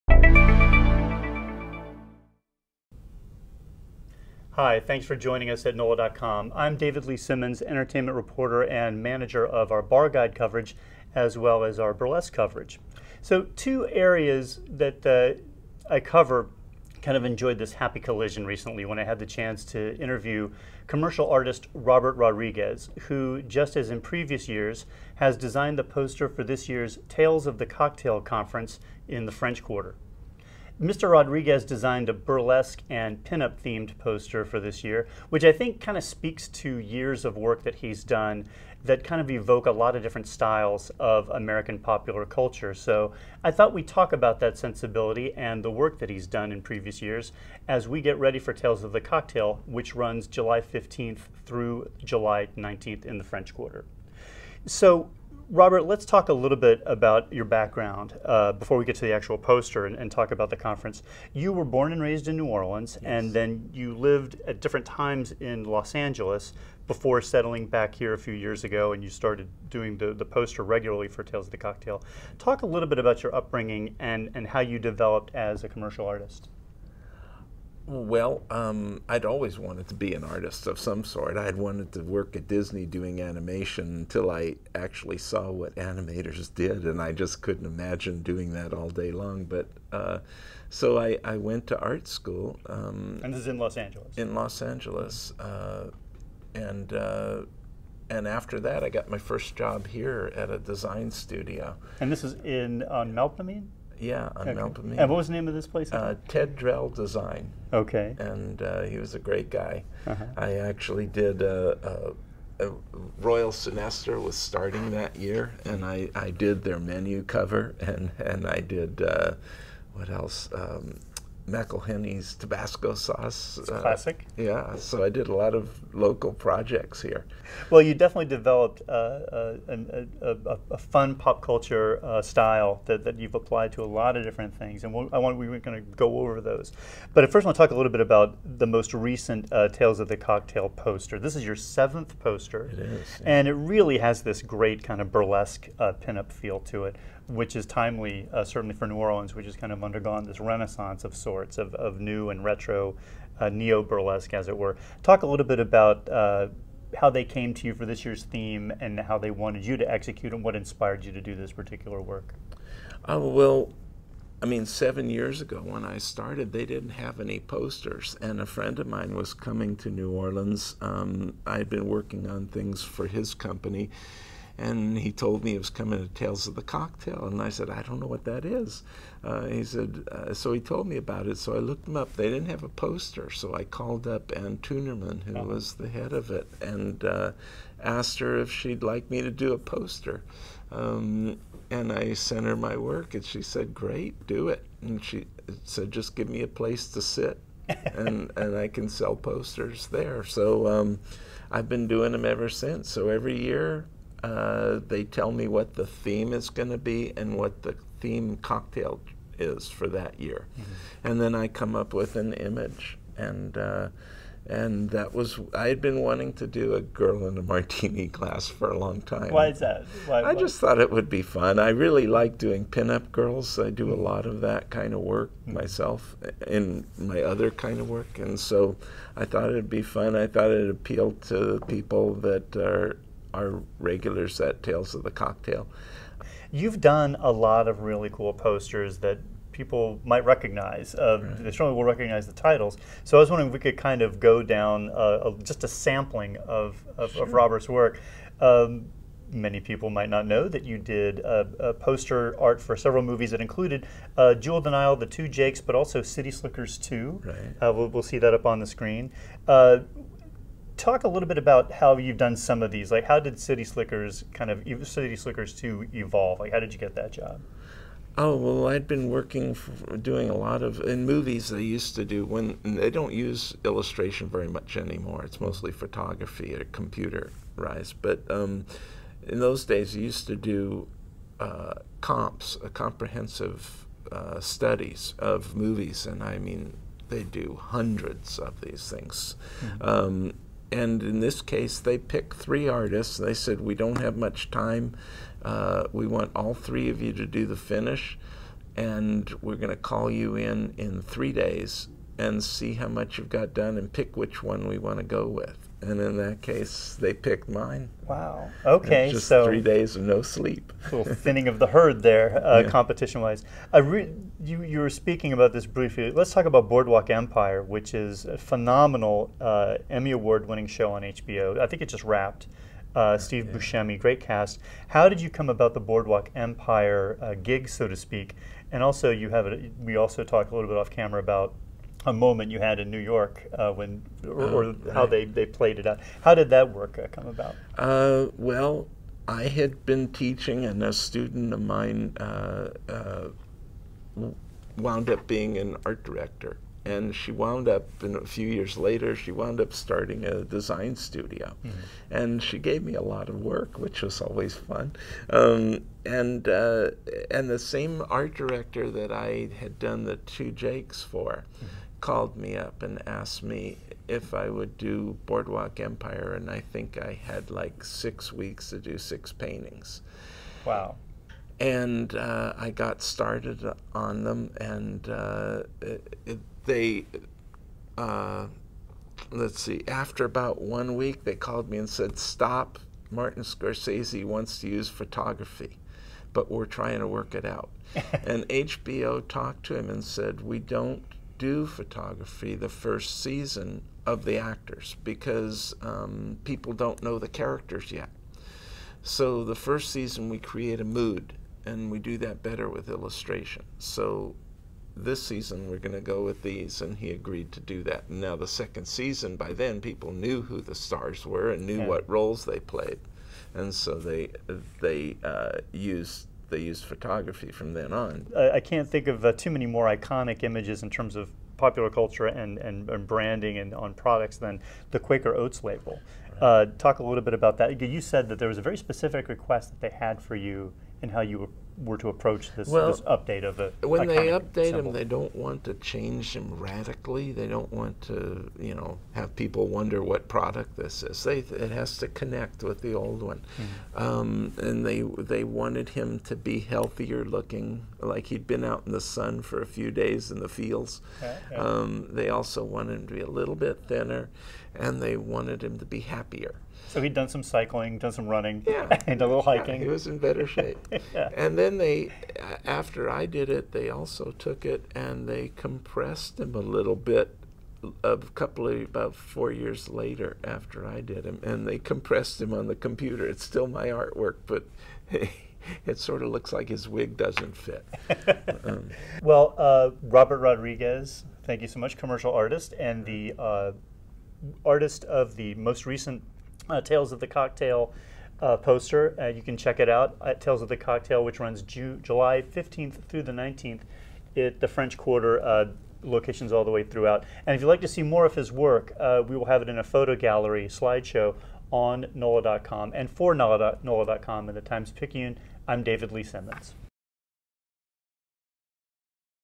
Tales of the Cocktail artist Robert Rodriguez
Robert Rodriguez, a nationally respected commercial artist and designer, has fashioned posters and other artwork over several decades -- including all of the posters for the annual Tales of the Cocktail conference in the French Quarter. Here he discu